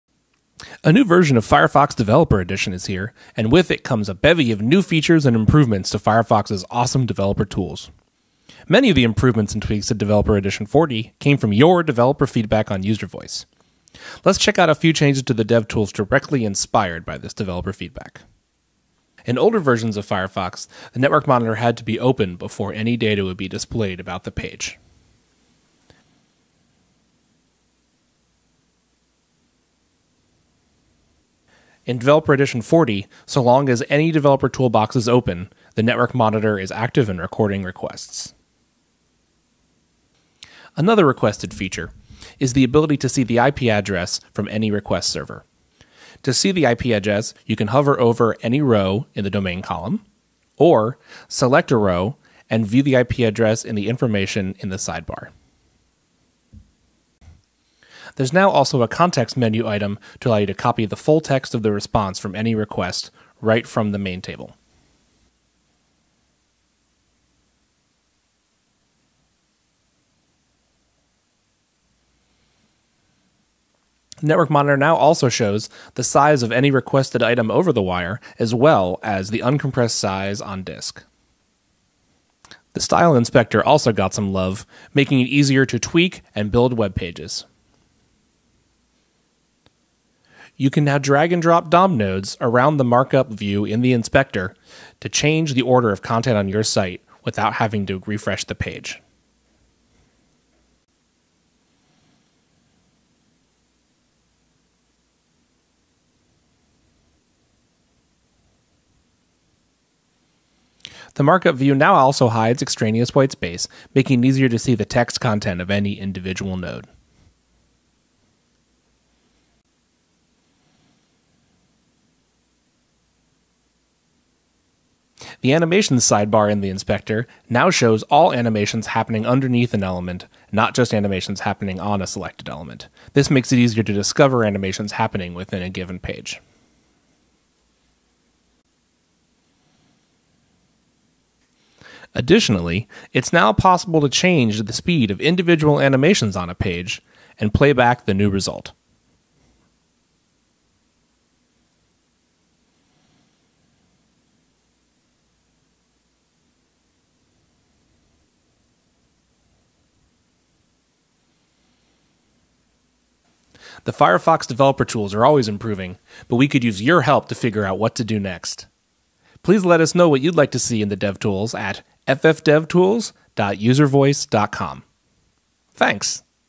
Usability improvement highlights in Firefox Developer Edition 40
This video demonstrates some of the new usability improvements in Firefox Developer Edition 40